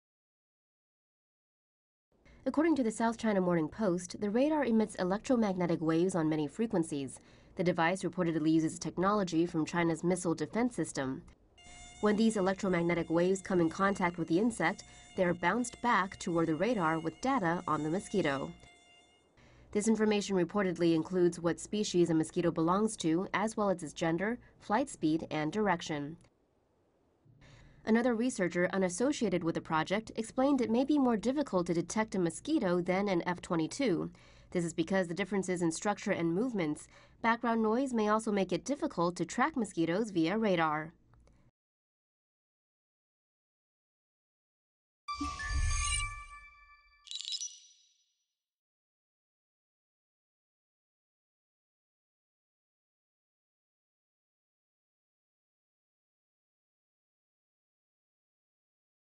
Radar can pinpoint mosquitoes from 1 mile away: Chinese scientists
New radar technology can track the flight path of a mosquito from a mile away, claim Chinese scientists.

The device uses technology from China’s missile defence system, reports the South China Morning Post, citing an unidentified researcher. A prototype is being tested at the Beijing Institute of Technology.

The device can reportedly identify which species a mosquito belongs to as well as its gender, flight speed and direction.

RUNDOWN SHOWS:
1. Depiction of radar sending electromagnetic waves
2. Depiction of electromagnetic waves bouncing off a mosquito
3. Depiction of radar receiving information
4. Depiction of radar with mosquito and F22

VOICEOVER (in English):

“According to the South China Morning Post, the radar emits electromagnetic waves on many frequencies.”

“The device reportedly uses technology from China’s missile defence system.”

“When these electromagnetic waves come into contact with the insect, they are bounced back toward the radar with data on the mosquito.”

“This information reportedly includes what species a mosquito belongs to as well as its gender, flight speed and direction.”

“Another researcher, unassociated with the project, explained it may be more difficult to detect a mosquito than an F-22.”

“This is because of differences in structure and movements. Background noise may also make it difficult to track mosquitos via radar.”